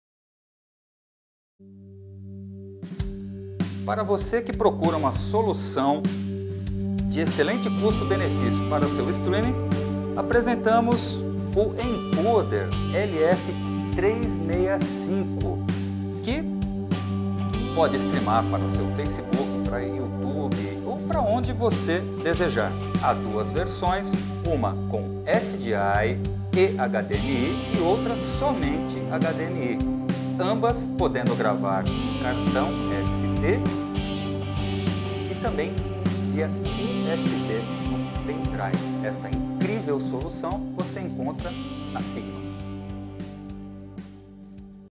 Encoder Seegma - Solução de baixo custo para fazer Streaming ao Vivo
Solução de baixo custo para fazer Streaming ao Vivo para plataformas como Facebook, Youtube e mais!